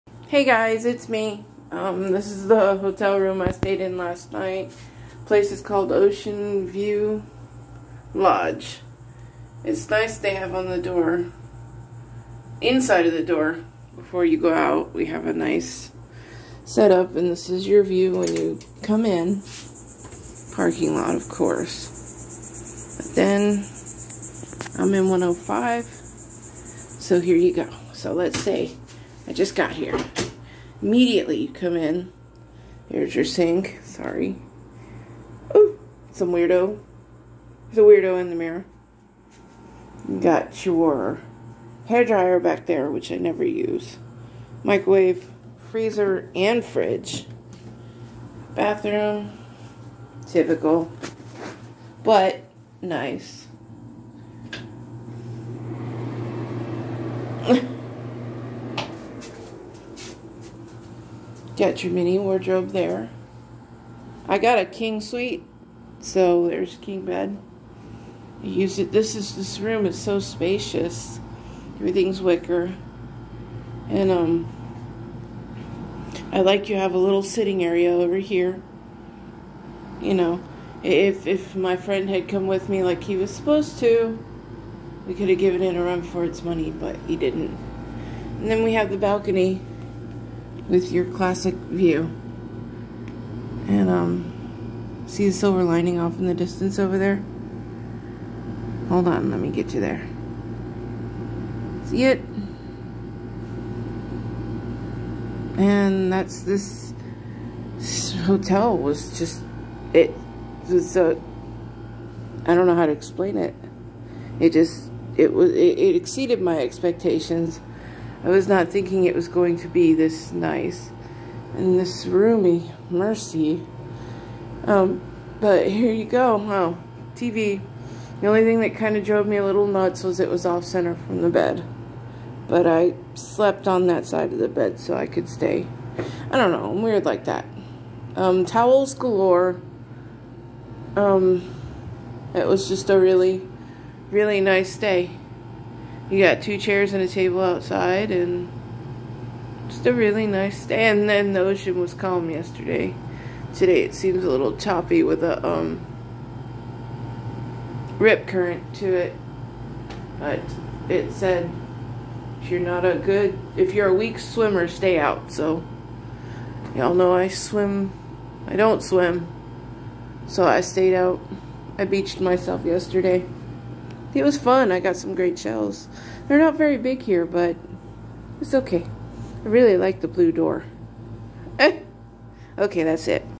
last hotel of my strike out on my own tour
it's called the ocean view lodge the title is not false advertising. I had a first floor room there were no rooms under mine. I don't like king size beds I have to get a running start to get up in it and I only use the space that I'm used to in a twin bed. my support friend ghosted me. this trip was for both of us but I did it alone. I call it the strike out on my own tour. for two years I had a ride or die pal. I dunno what happened but no refunds it had to be done.